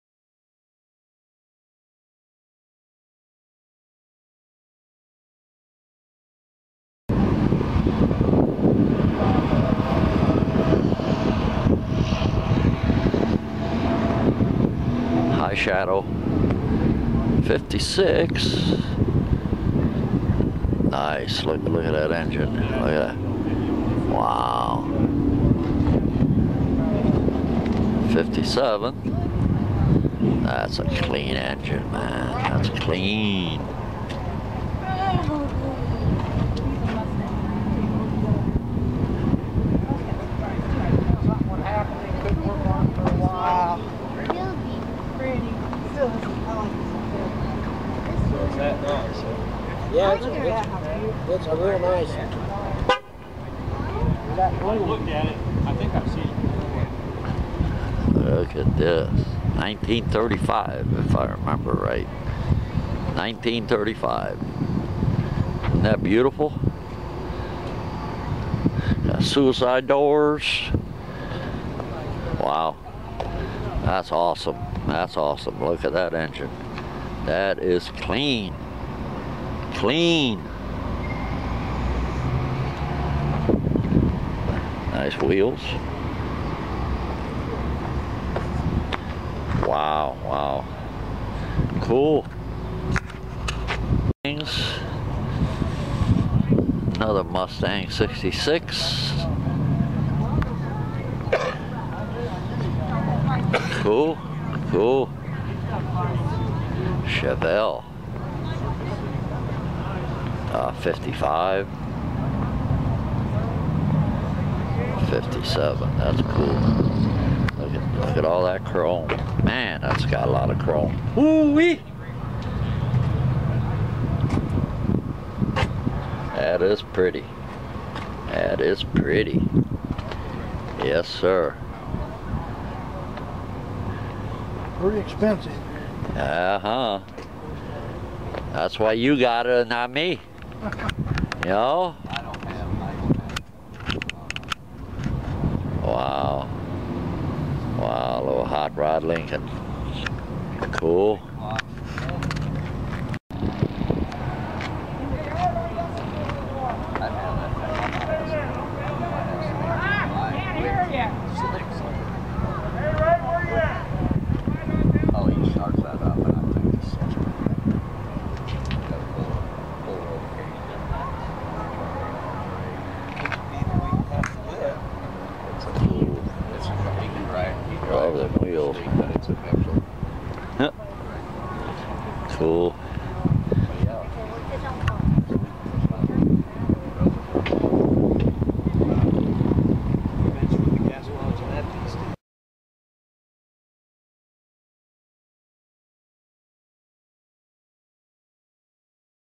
Car Show (Wabash, Indiana)
A car show in Wabash, Indiana on August 6th, 2013